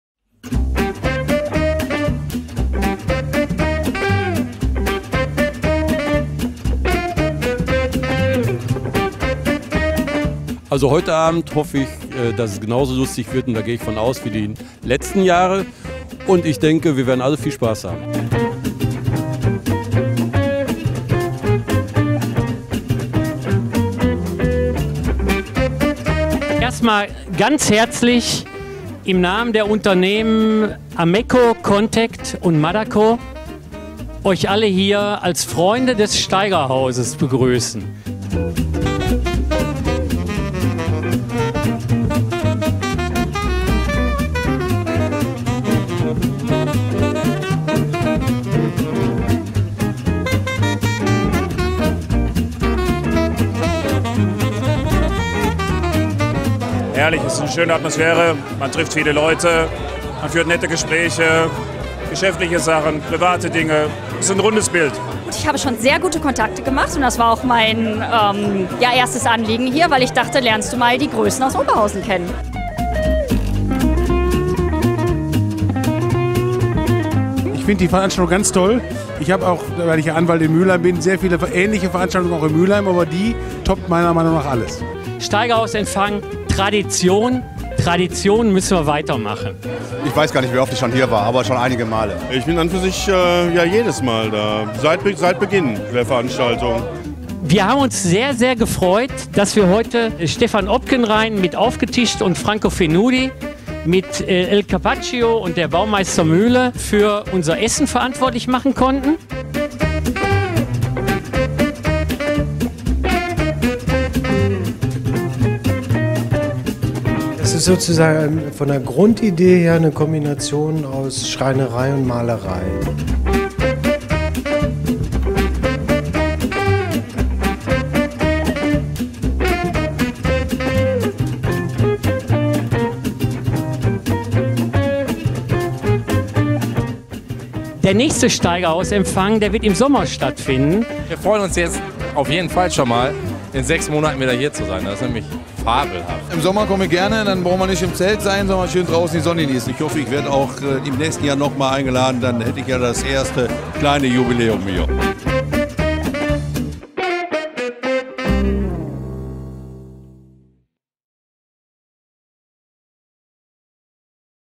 Steigerhaus Empfang 2014
Der gemeinsame Empfang der Oberhausener Agenturen Contact GmbH und AMECKO sowie des Architekturbüros MADAKO hat Tradition.
Bereits zum 15. Mal trafen sich Kunden und Freunde im GeckoTorhaus gegenüber des Steigerhauses der ehemaligen Zeche Osterfeld, dem Firmensitz der Gastgeber.